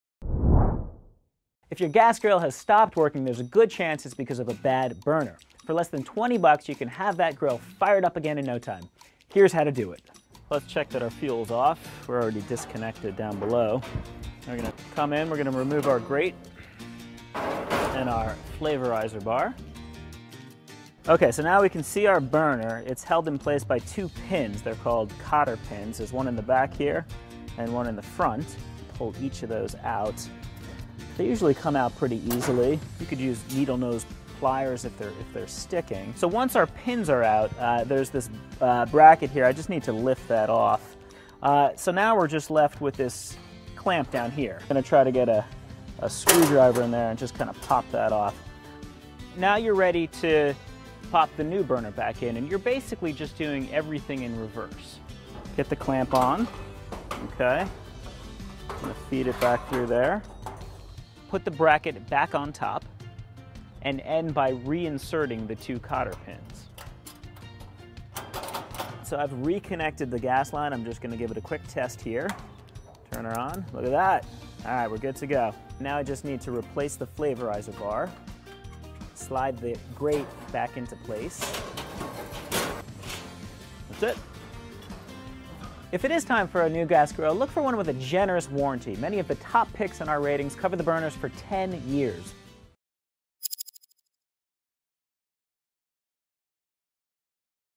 How to Replace a Gas Grill Burner | Consumer Reports
Before you kick your broken gas grill to the curb, check the burners. A faulty burner is often the reason a grill won't work, and the fix is easy. Consumer Reports shows you how.